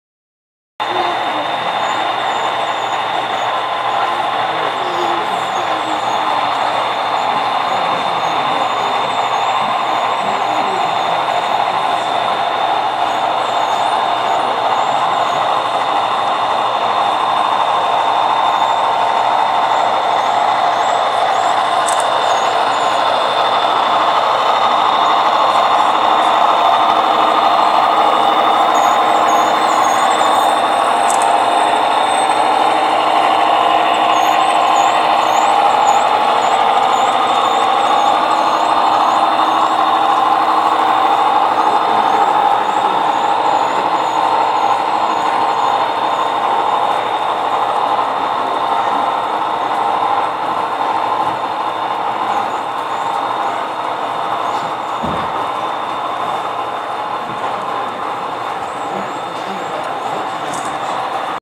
Chi Town Union Station
Home To the World's Largest O Scale Model
Railroad and Longest Model train.